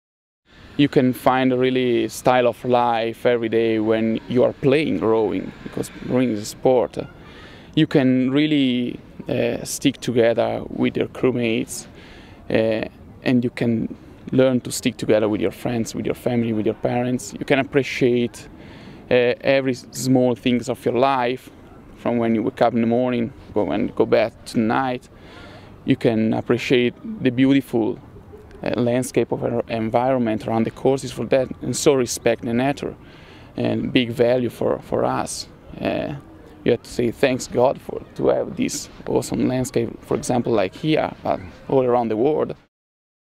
Niccolo Mornati (ITA) - What does rowing bring to your life?
Rowing athlete Niccolo Mornati of Italy talks about the rowing lifestyle.